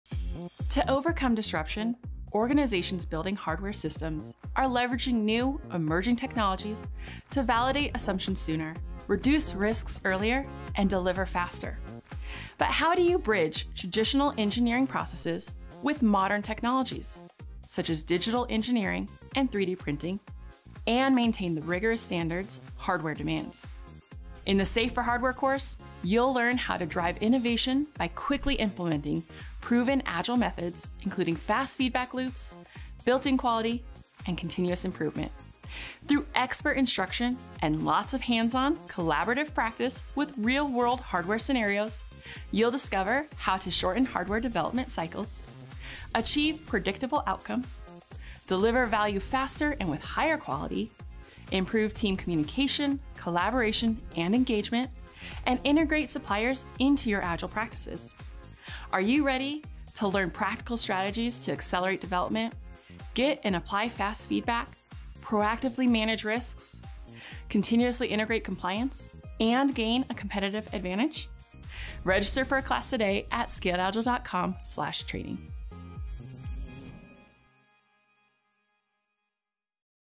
Accelerate Hardware Design & Delivery with SAFe® for Hardware
Are you ready to learn practical strategies to accelerate development, achieve predictable outcomes, deliver value faster with higher quality, and improve team communication, collaboration, and engagement?

Bridge traditional engineering processes with modern technologies with agile practices. Explore the course